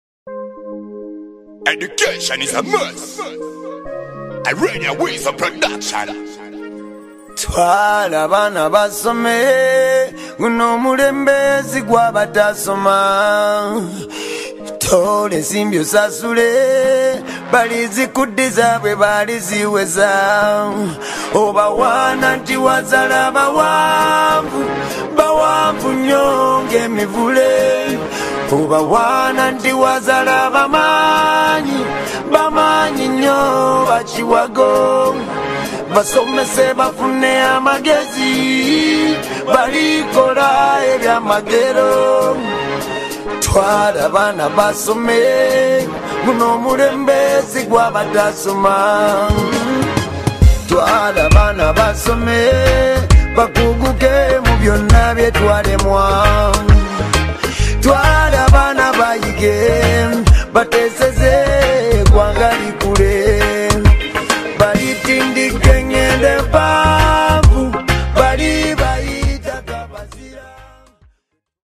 Welcome to Dayton Junior School
Dayton Junior School is an international school located along Masaka Road Maya - Nanziga a few kilometers from the capital city of Uganda Kampala
It's divided into two sections, daycare and boarding section with a well-established environment that favors easier learning for every student in each platinum